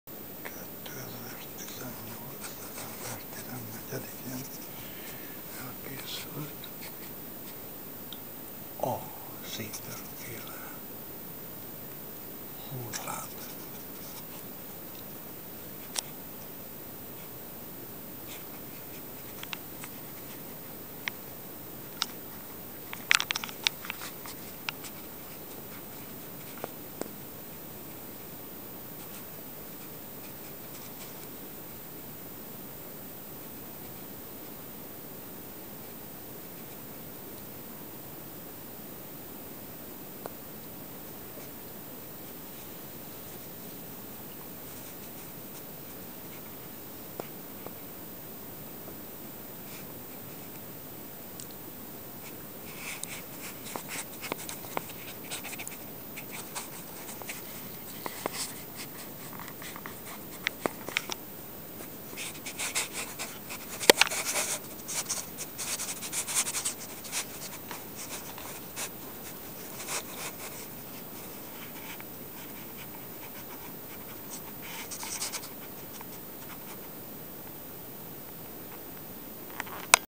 SEEGER HÚRLÁBAM 1
A hosszú nyakú bendzsómra elkészült a Pete Seeger típusú  húrlábam.